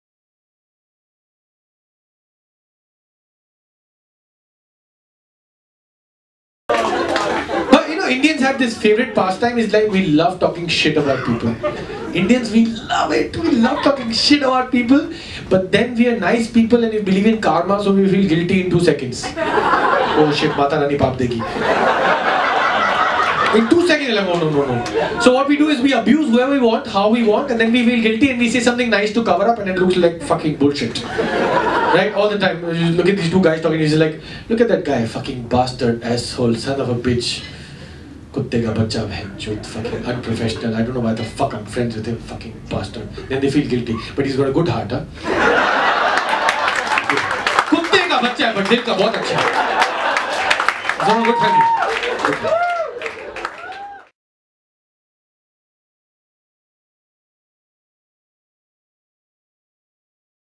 INDIANS HAVE A GOOD HEART I STAND UP COMEDY BY Nitinn R Miranni
Mr Komic Sutra - Nitinn R Miranni talks about how Indians have a Good Heart

Watch n Share